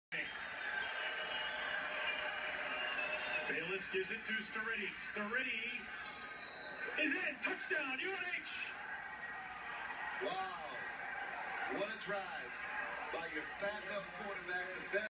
UNH football vs Richmond 2014
UNH TD with seconds left